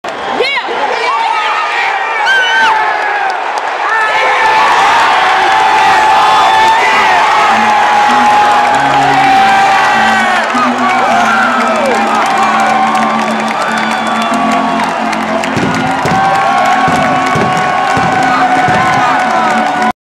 White Sox's Beckham's walk off for the win
This is so short because my camera used up its harddrive disk space right when the team ran to congratulate Beckham.